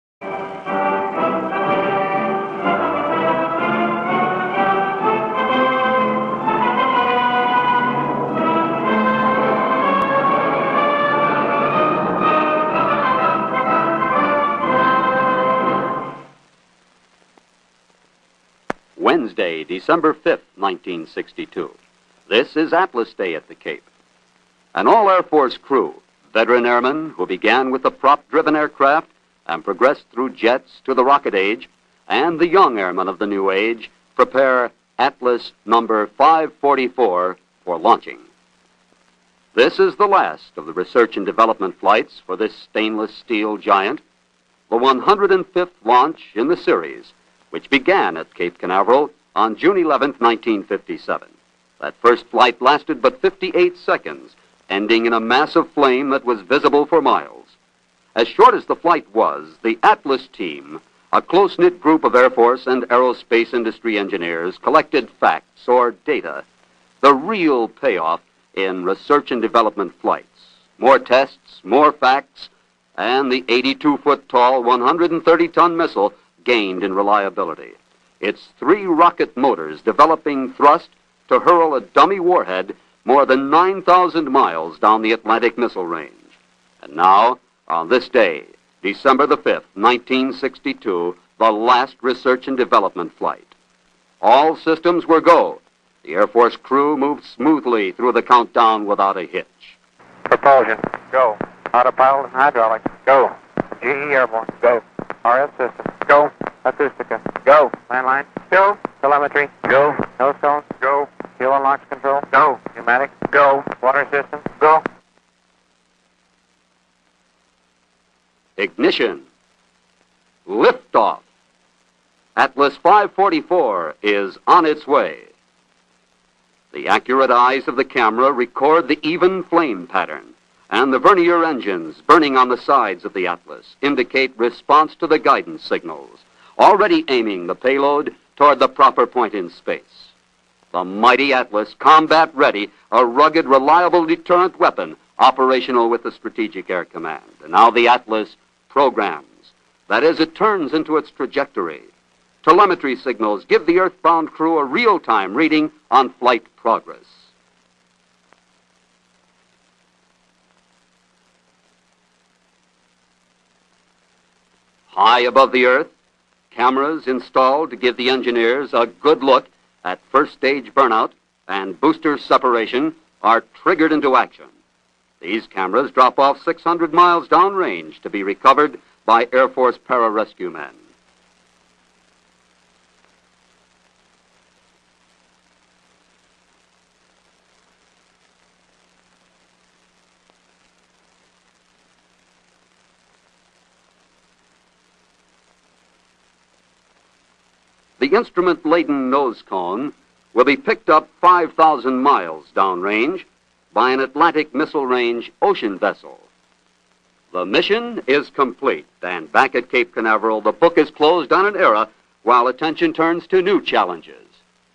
1962: Atlas 544
The last test flight of the Atlas rocket from Cape Canaveral.

Credit: NASA